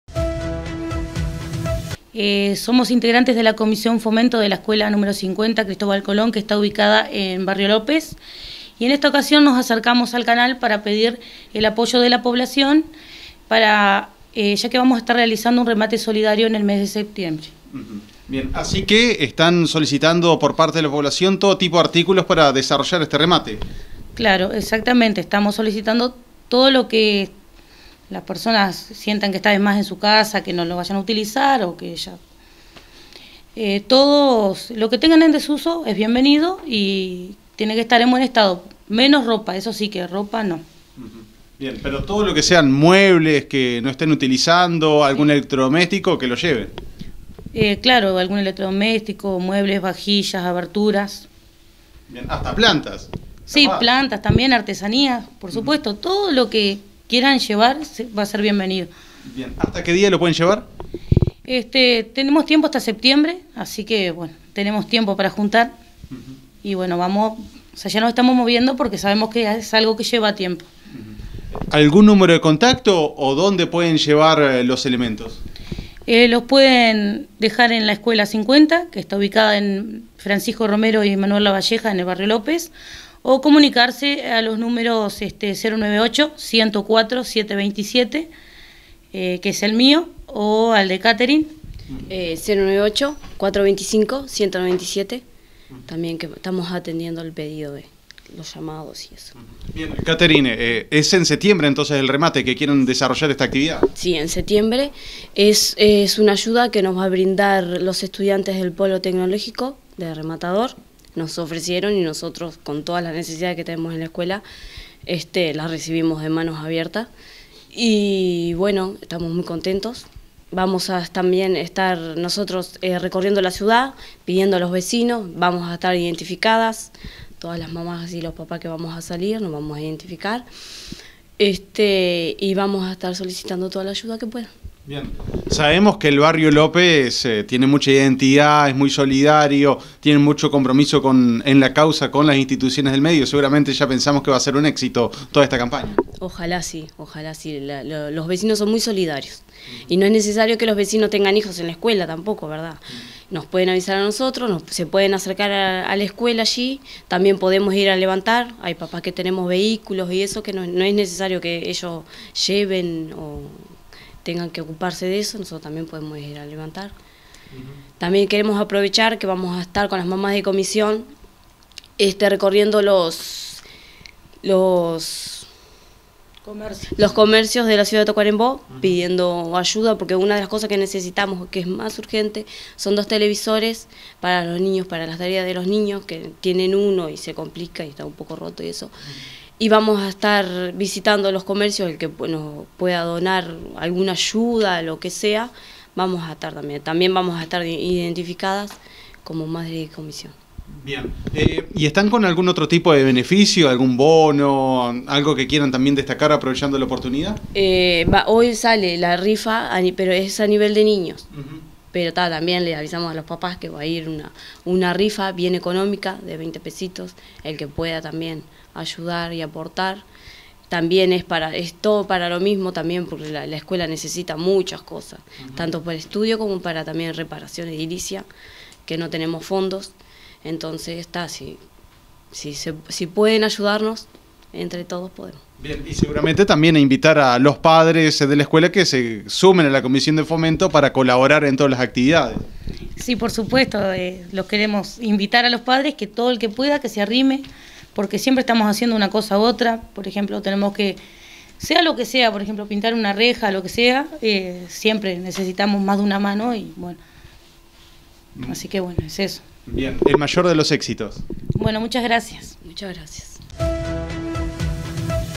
Remate Solidario en barrio López
La Comisión Fomento de la Escuela 50 del barrio López, está recolectando diferentes materiales para concretar un remate solidario en setiembre.